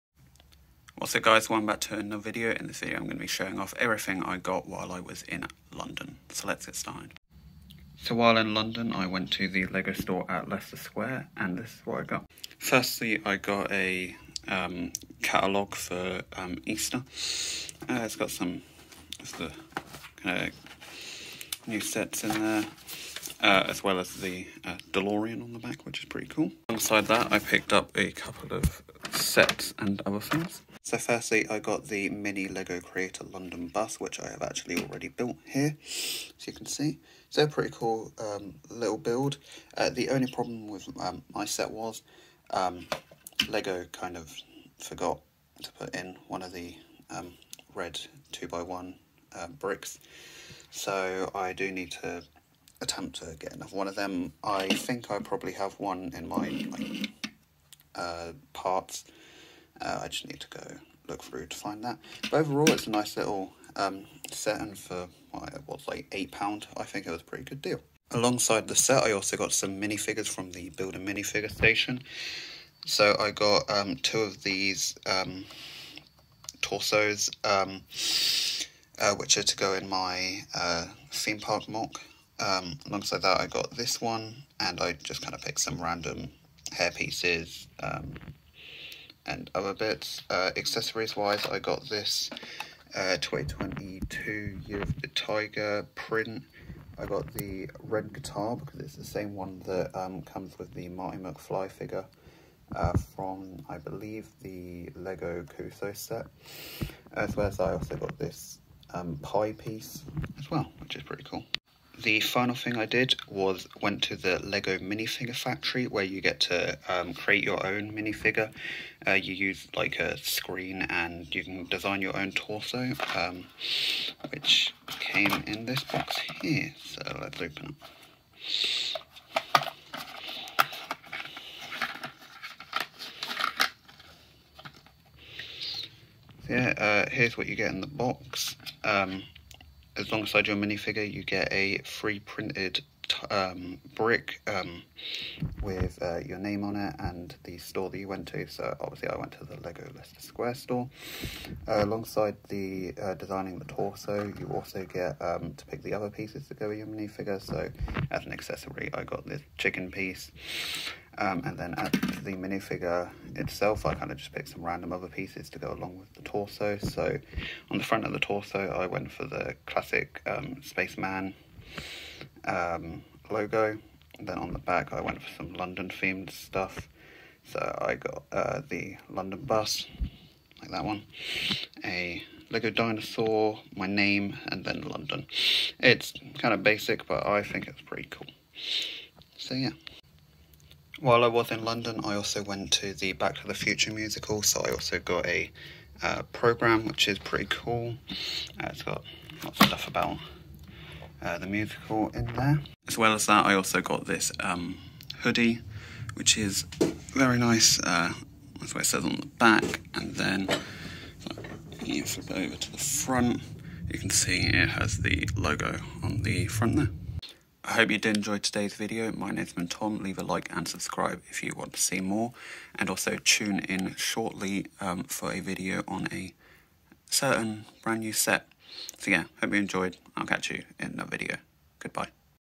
LEGO Shopping In London | Leicester Square + Back To The Future Musical
Hi All,
Welcome to another video. In this video I show off all the sets and minifigures I picked up at the Leicester Square Lego Store as well as the stuff I bought at the Back To The Future Musical!